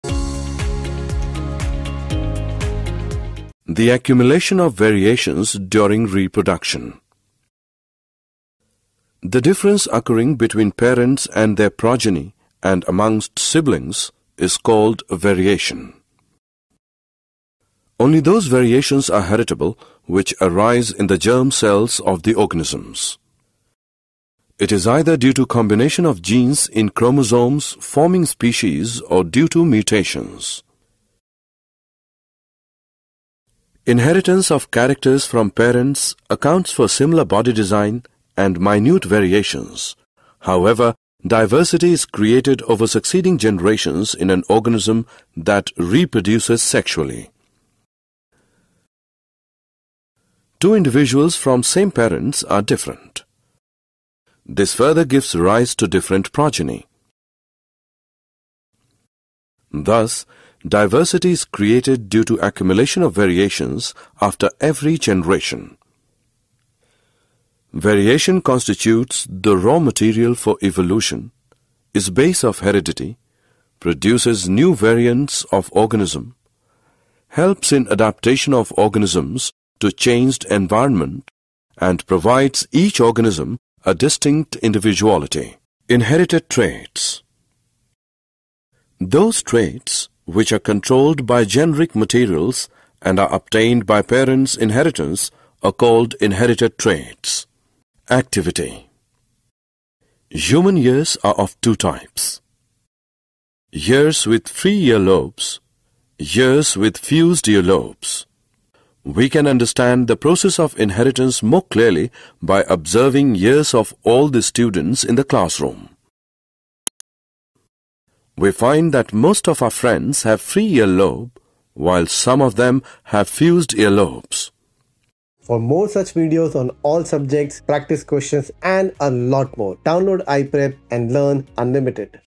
The Accumulation of Variations During Reproduction | Heredity and Evolution | Science | Class 10th
In this video, we have explained The Accumulation of Variations During Reproduction, an important concept in the chapter, Heredity and Evolution from Science  for Class 10th.

iPrep offers rich and comprehensive learning content for Nursery to 12th including:

✅ Chapter-Wise Assessments
✅ Co-curricular Courses
✅ Career Orientation